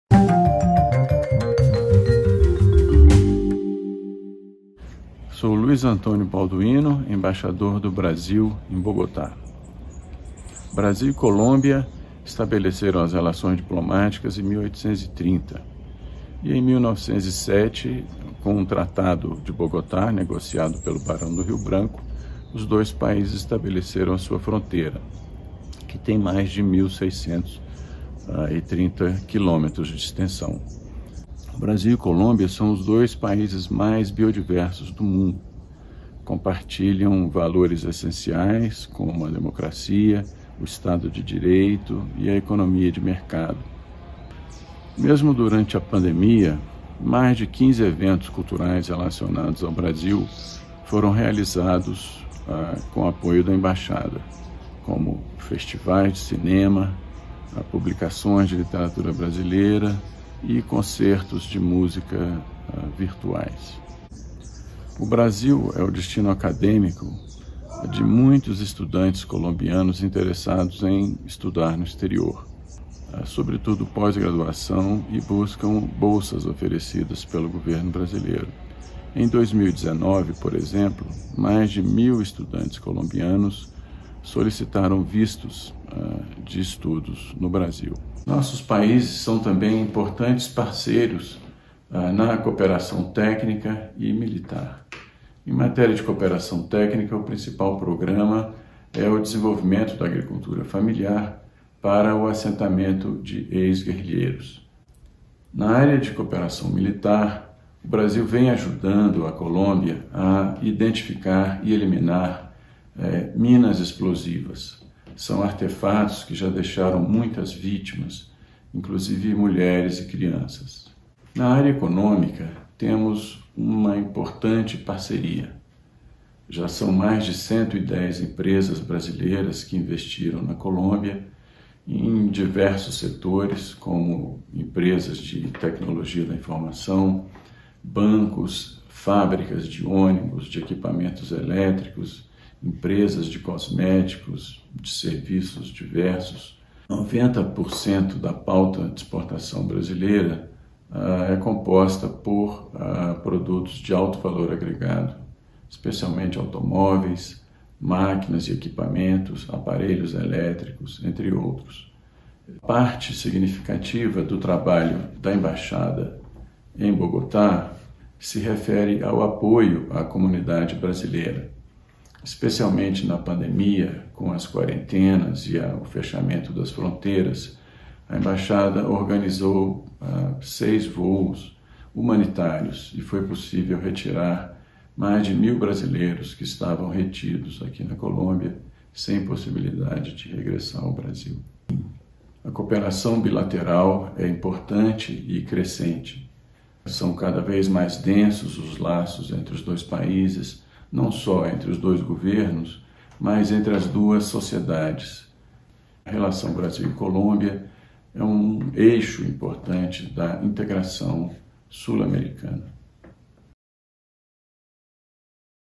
Relações Bilaterais Brasil-Colômbia
Vídeo do Embaixador do Brasil em Bogotá, Luís Antonio Balduino Carneiro, sobre as relações entre o Brasil e a Colômbia.